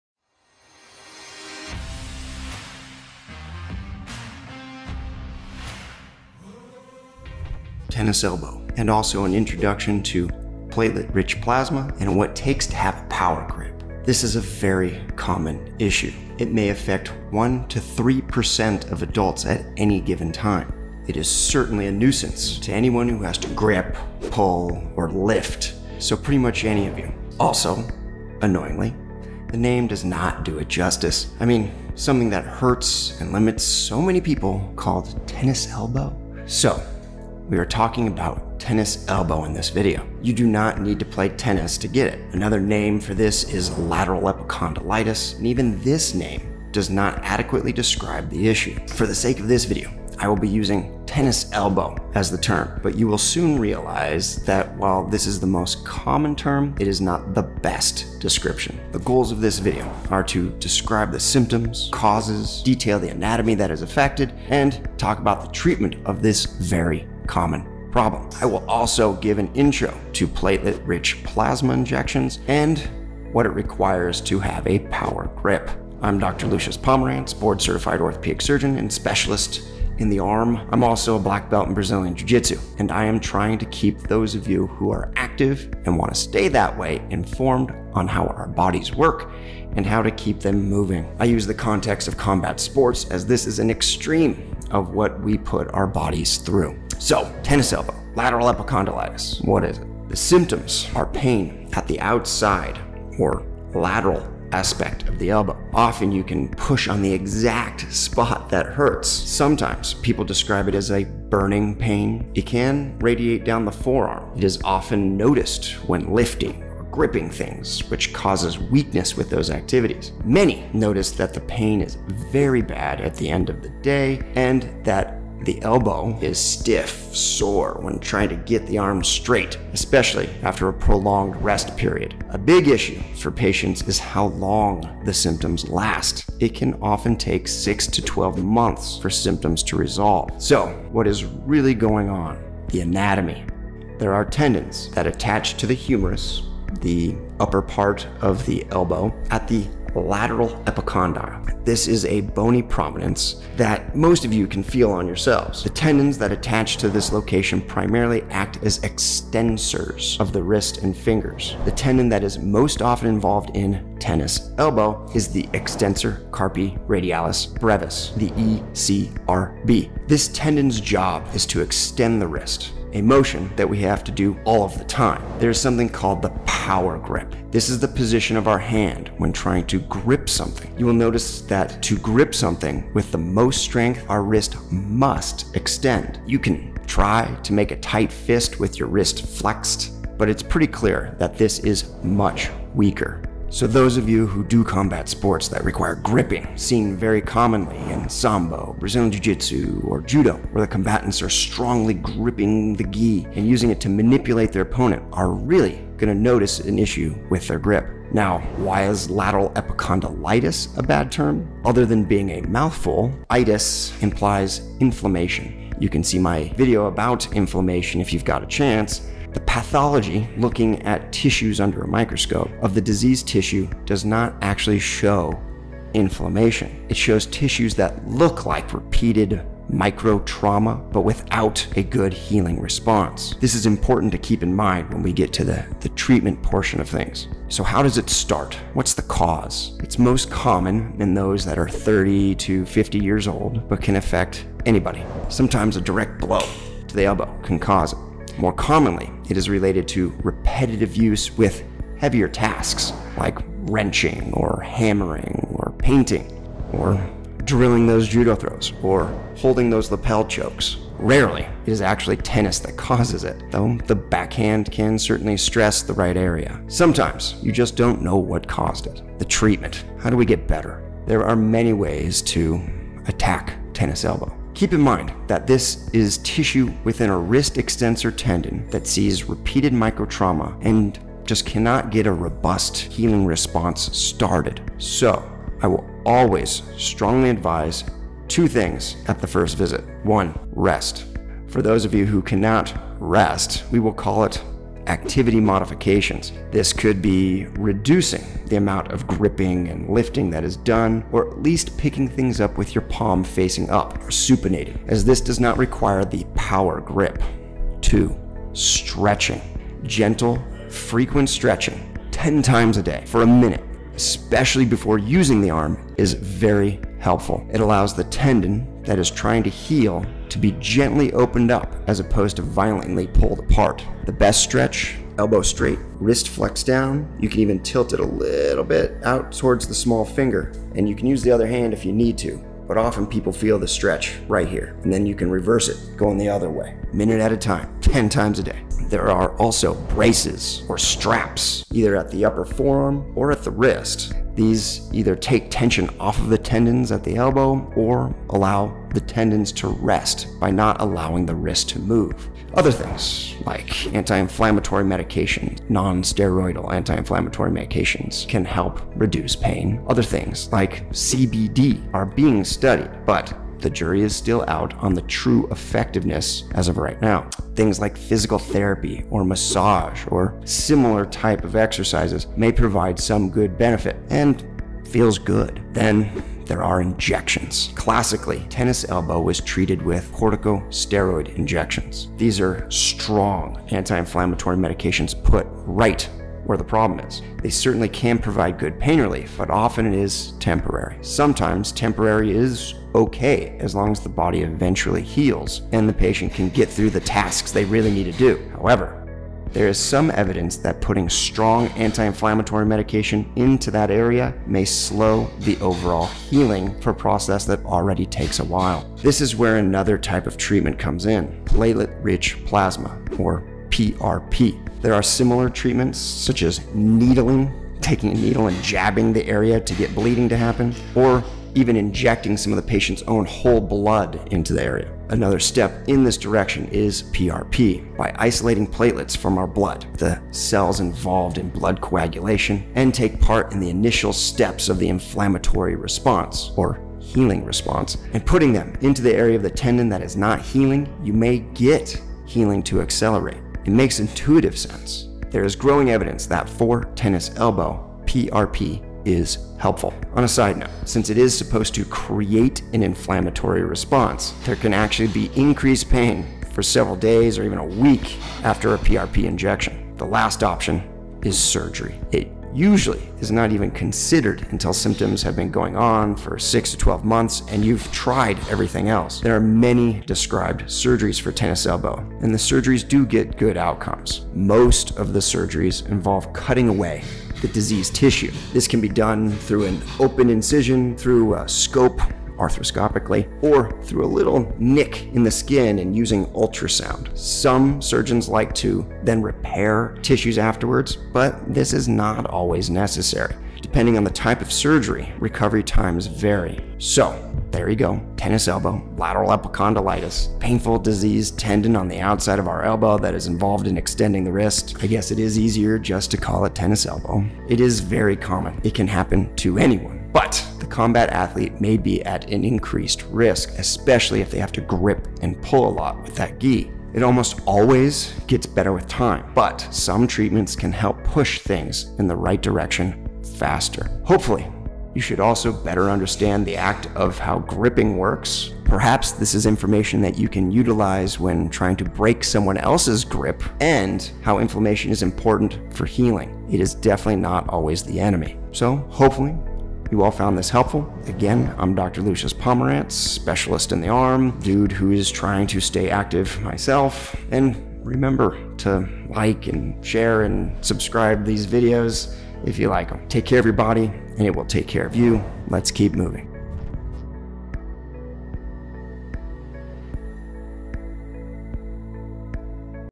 Tennis Elbow (also an intro to platelet rich plasma or PRP and what it takes to have a power grip…)
Tennis elbow is a very common issue. It may affect 1-3% of adults at any given time. It is certainly a nuisance to anyone who has to grip, pull, or lift – so pretty much any of you. Also, annoyingly, the name does not do it justice. Another name for this is lateral epicondylitis, but even this name does not adequately describe the issue.

The goals of this video are to describe the symptoms, causes, detail the anatomy that is affected, and talk about the treatment of this very, very common problem. I also will give an intro to platelet rich plasma (PRP) injections and what it requires to have a power grip.

0:00 Introduction
1:37 Symptoms of Tennis Elbow
2:23 Affected Anatomy in Tennis Elbow
2:55 Power grip
3:58 Causes of Tennis Elbow
4:28 Treatments for Tennis Elbow
6:51 Platelet-Rich Plasma Injections and Tennis Elbow
8:45 Conclusion
If you have an idea of something you want  to see me cover on this channel please feel free to let me know!